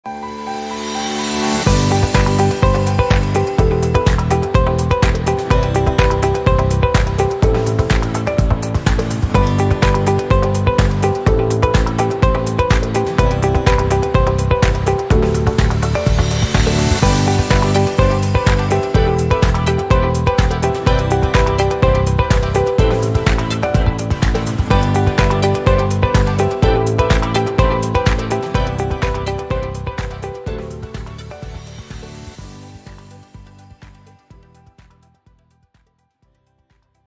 4 Tier Cart
【Item: BC363】
Simple double side frame with removable storage box, plus the hanging pipes on the side which makes the storage more flexible.

Feature:
✔Side frame can be used as both handle & hanging objects.
✔Removable shelf makes storage more convenient.